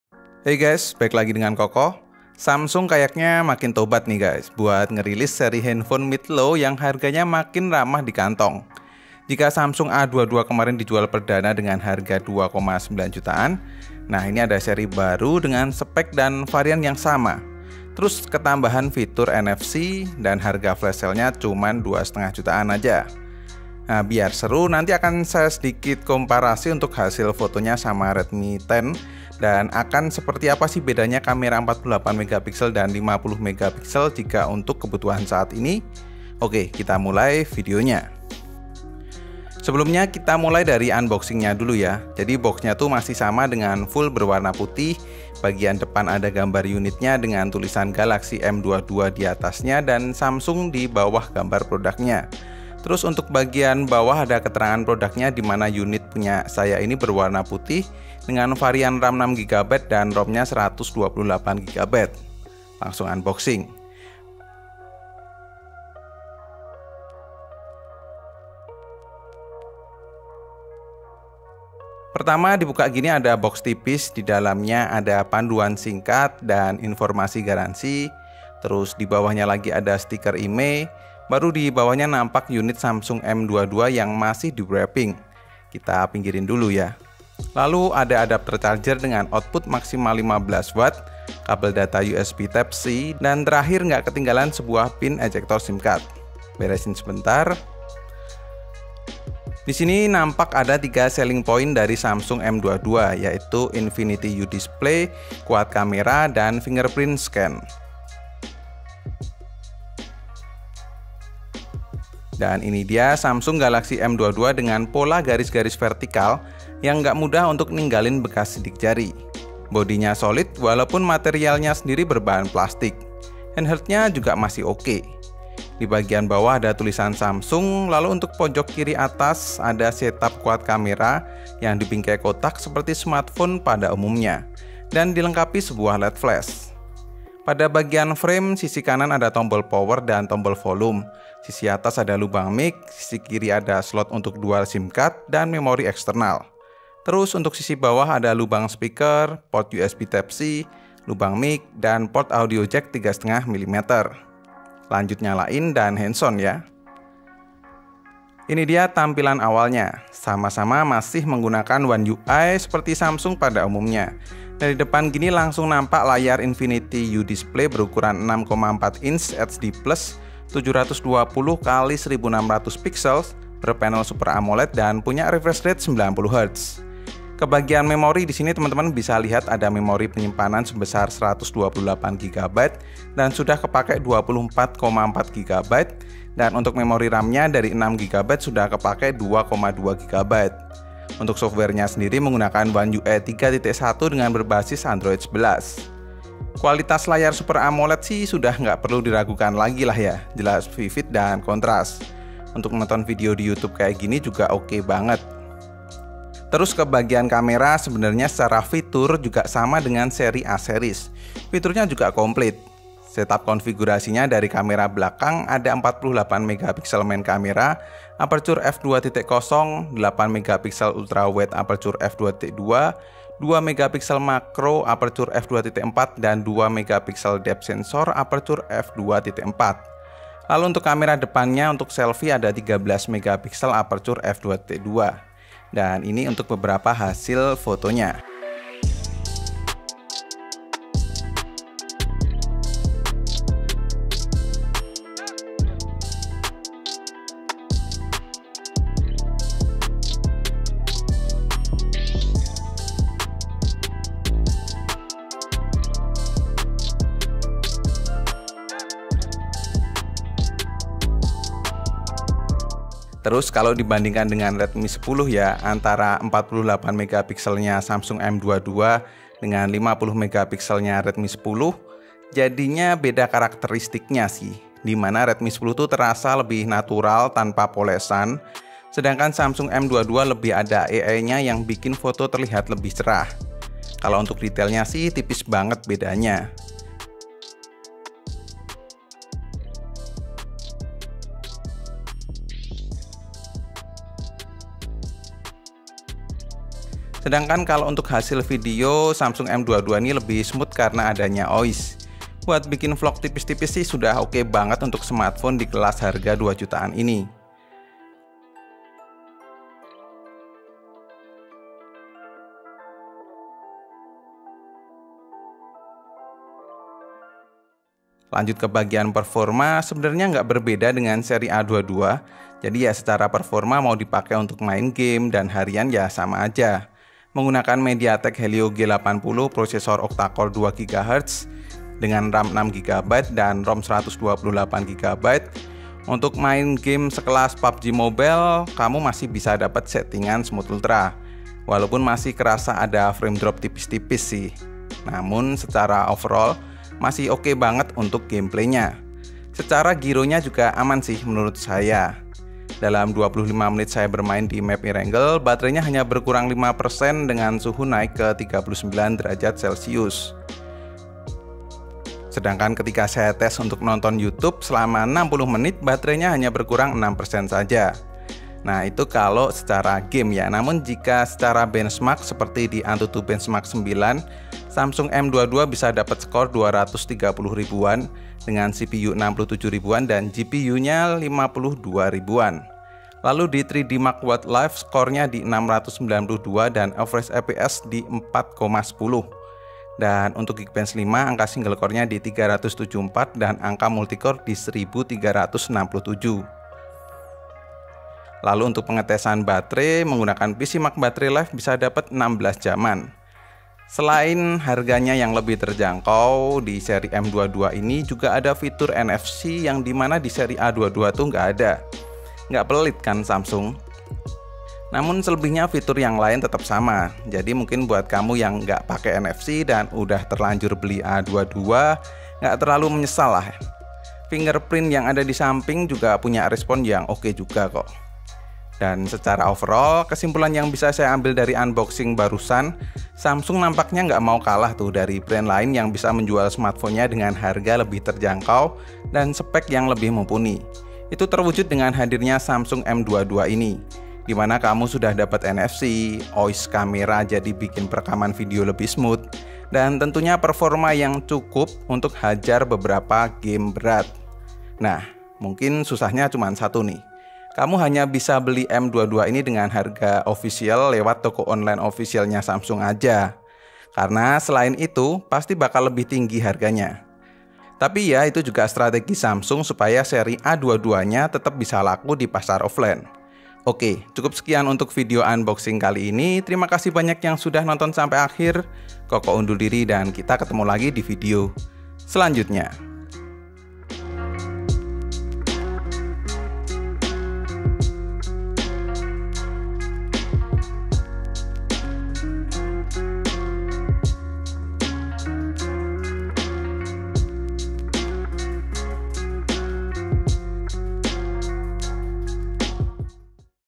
Samsung M22, sama 2jutaan mending mana dibanding Redmi 10
Pasar 2jutaan makin rame aja neh!!